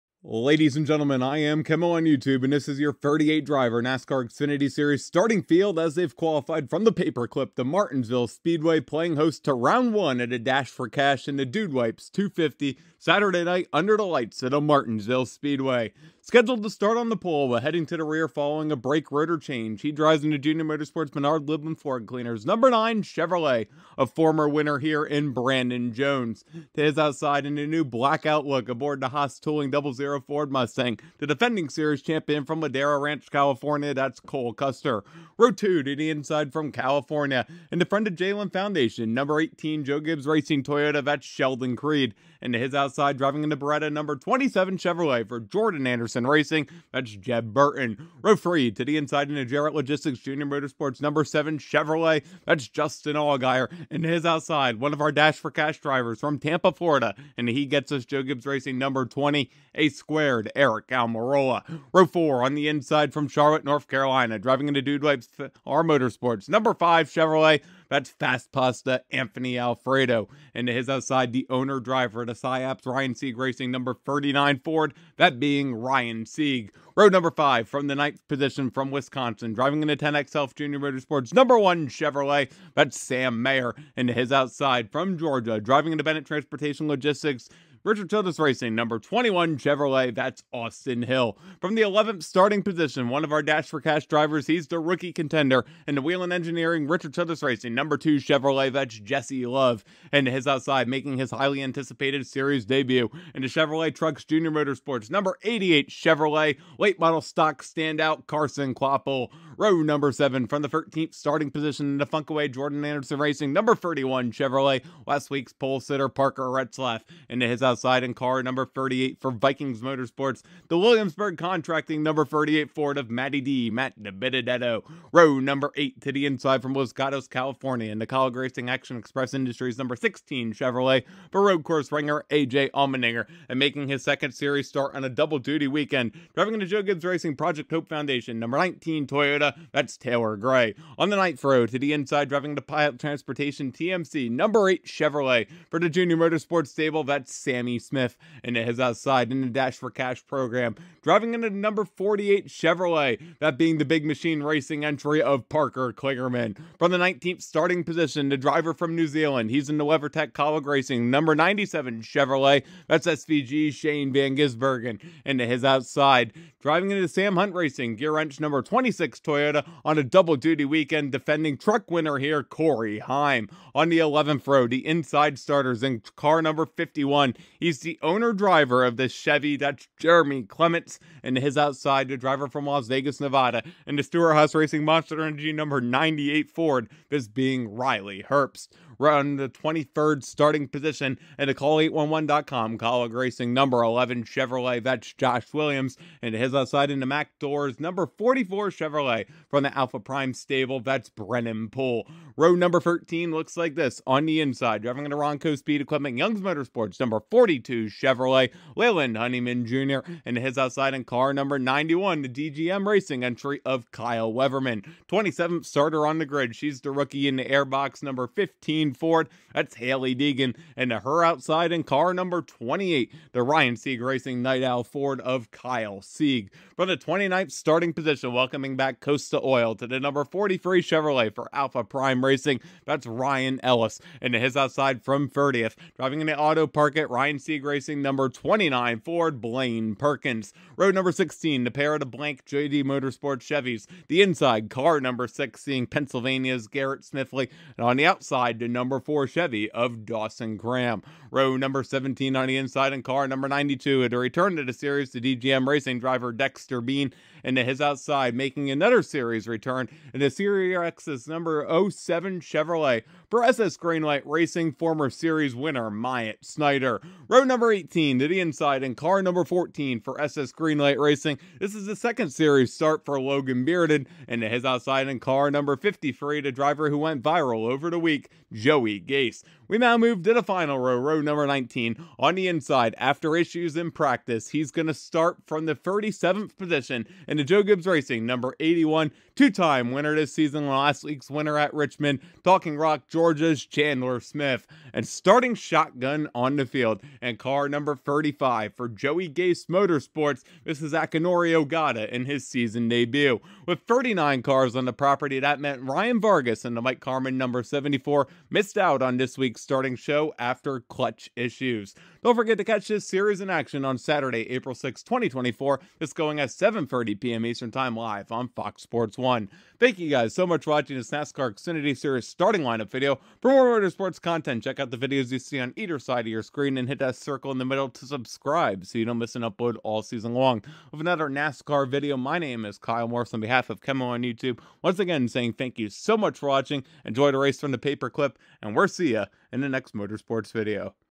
2024 Dude Wipes 250 at MARTINSVILLE | NASCAR Xfinity Series STARTING LINEUP
The Virginia short track action continues for the series where names are made as the NASCAR Xfinity Series stars descend on the so-called "paperclip", the Martinsville Speedway for 250 laps of action. Today we look at your full 38 driver starting field ahead of the Dude Wipes 250 at Martinsville.

CREDITS:
All NUMBER GRAPHICS shown in this video are credit of Diecast Charv and are used in compliance with creative commons copyright laws.